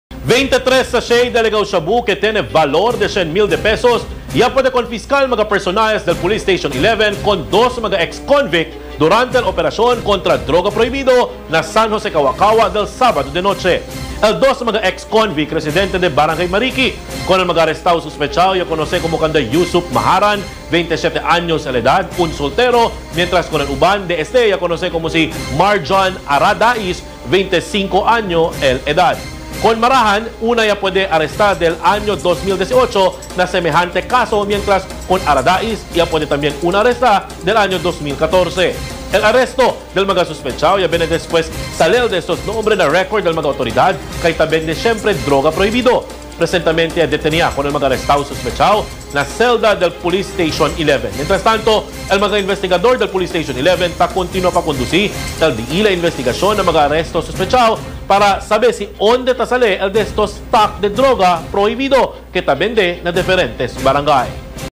ANTI-DRUG OPERATION
23 sachets of alleged Shabu worth P 100,000 was confiscated by Police Station 11 personnel from 2 ex-convicts during their operation at San Jose Cawa-Cawa Saturday Night.

Watch us live:
landline: 955-4324 or 926-3515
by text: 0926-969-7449